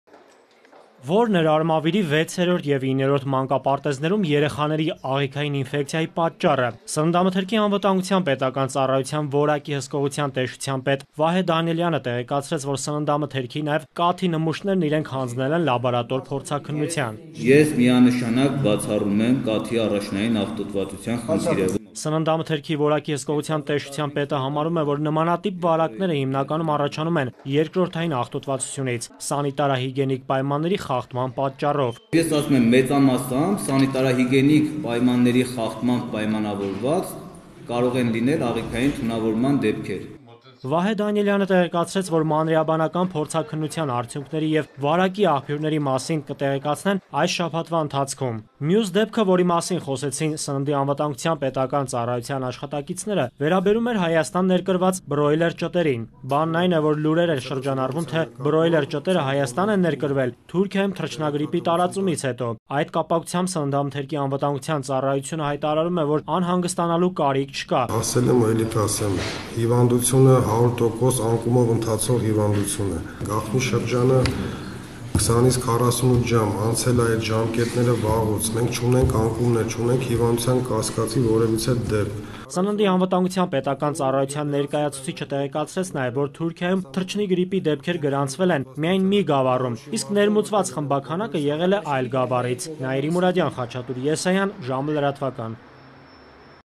Ո՞րն է Արմավիրի մանկապարտեզներում աղիքային ինֆեկցիայի պատճառը
ՍԱՊԾ-ն հրապարակել է առաջին եզրակացությունը Արմավիրի մանկապարտեզներում աղիքային ինֆեկցիայի զանգվածային վարակի պատճառի մասին: